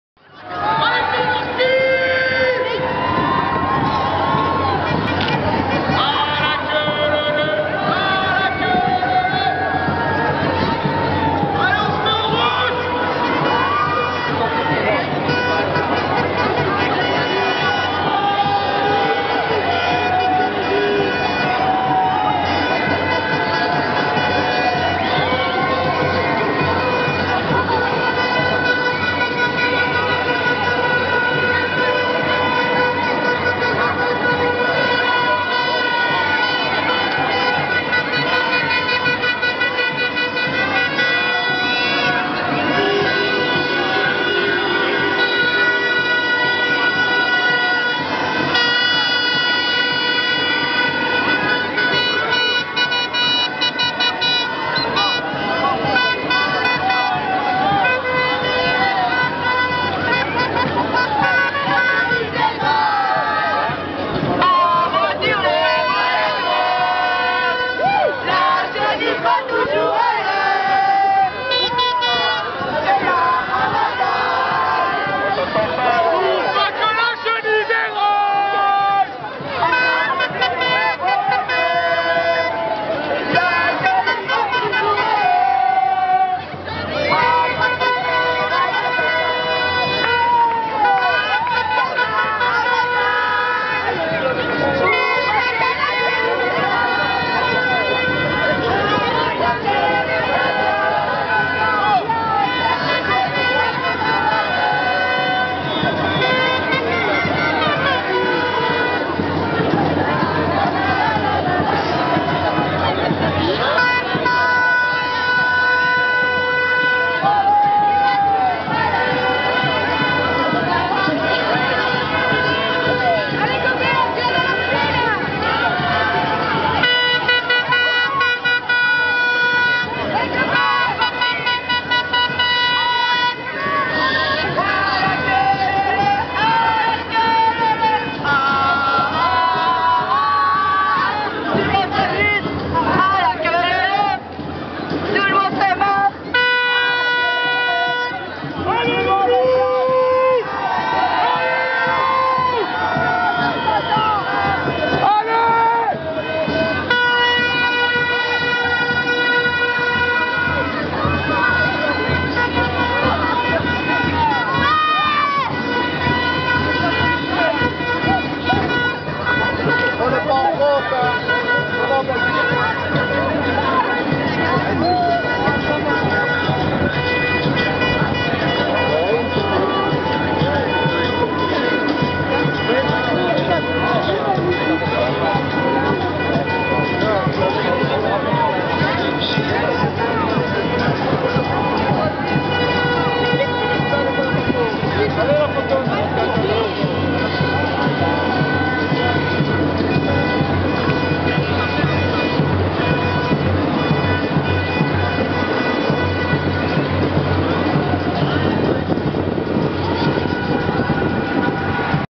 Fiers d'être Louviérois
Tu es un vrai louviérois si... tu fais une chenille géant dans le centre-ville. Un défi réussi le samedi 15 mars 2014 à La Louvière.